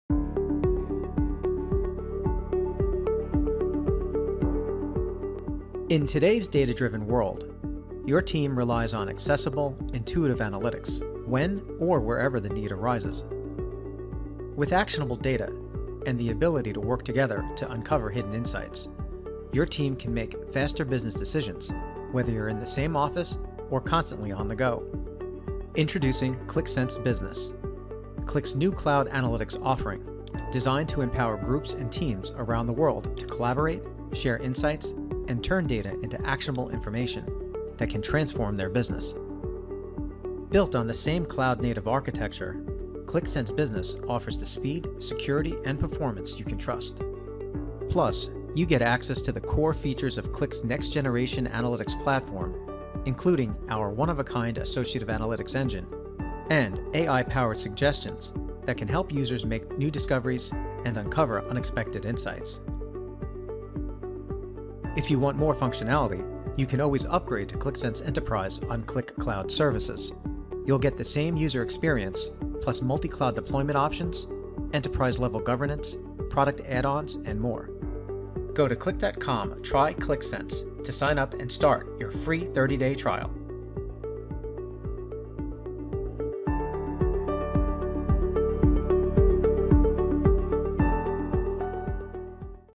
Introducción a Qlik Sense Business | Beneficios de esta herramienta de BI
En este breve video podrás descubrir todo lo que puede ofrecerte Qlik Sense. Dispondrás de análisis de tus datos de forma accesible en una plataforma intuitiva y de gran usabilidad.